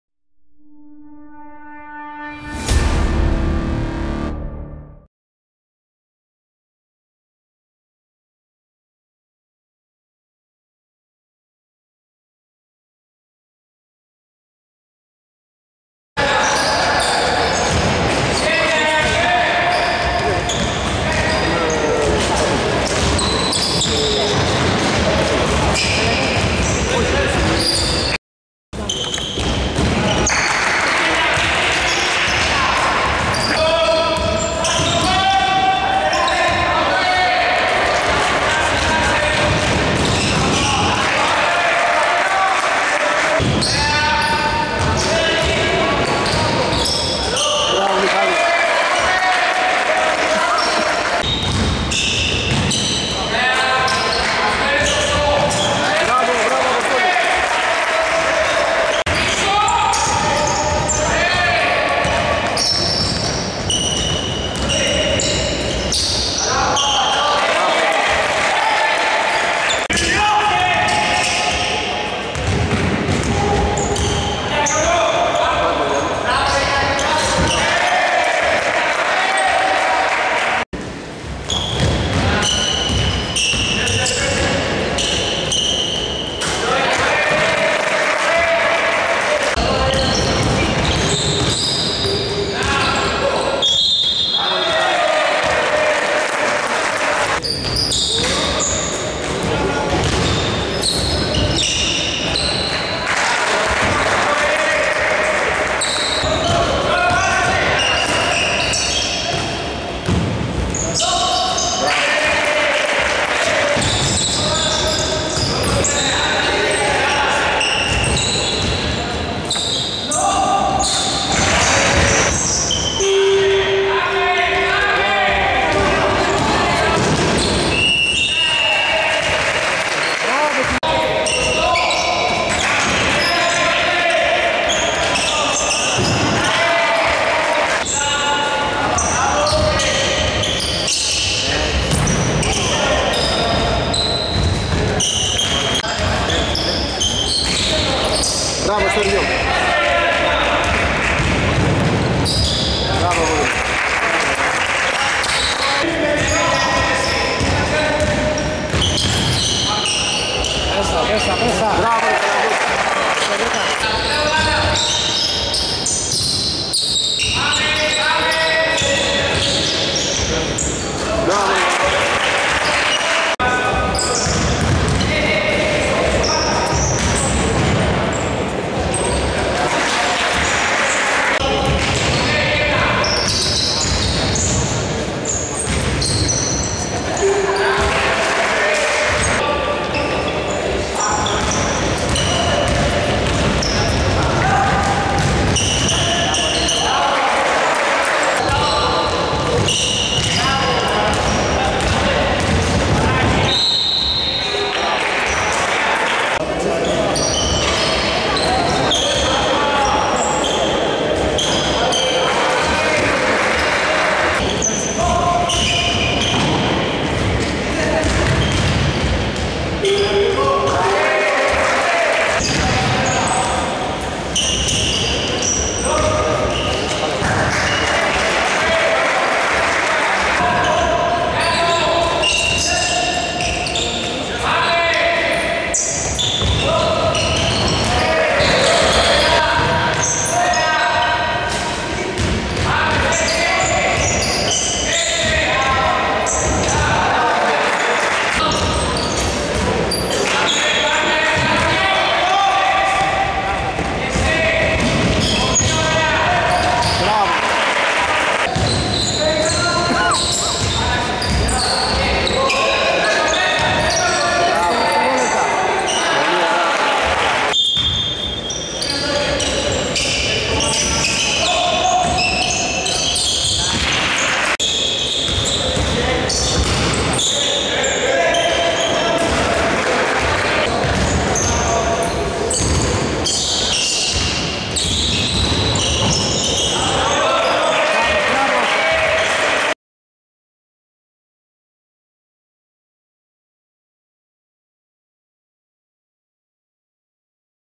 ΙΩΝΙΚΟΣ - ΗΛΕΚΤΡΟΥΠΟΛΗ 71-29
13η Αγωνιστική  - ΕΣΚΑΝΑ  Β.ΠΑΙΔΩΝ -

ΚΑΛΗ ΧΡΟΝΙΑ  σε όλα τα παιδιά και στο προπονητικό team, με υγεία ευτυχία χωρίς τραυματισμούς. Την Τρίτη στη Γλυφάδα αρχίζει η τελική ευθεία της χρονιάς, συνεχίζουμε το 2019 όπως τελείωσε το 2018 με συγκέντρωση και πάθος.